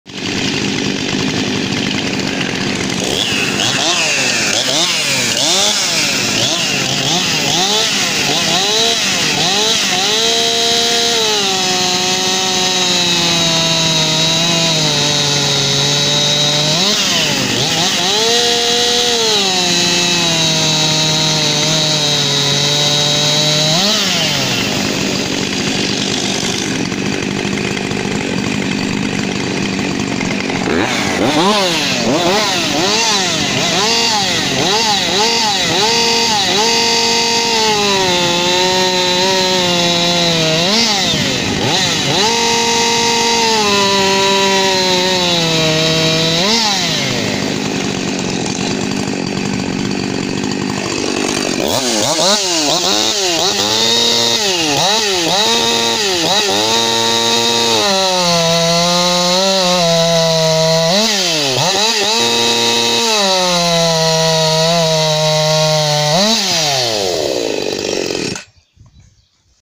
Stihl 261 vs husky 550 mark 2 vs husky 346 ported
husqvarna 346xp ported   50.1cm3  3.6hp
husqvarna 550 xp mark2  50.1cc.     4hp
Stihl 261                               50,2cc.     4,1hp